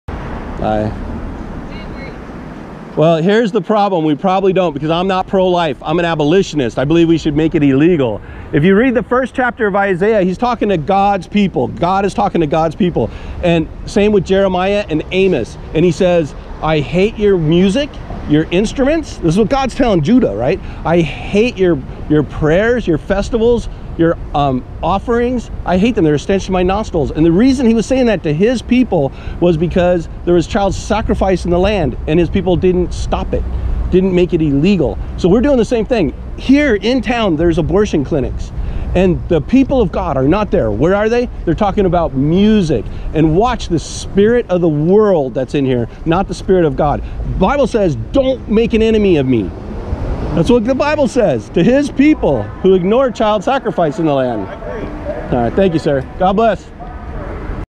We Agree with Abolitionism
Preaching abolitionism at the sing conference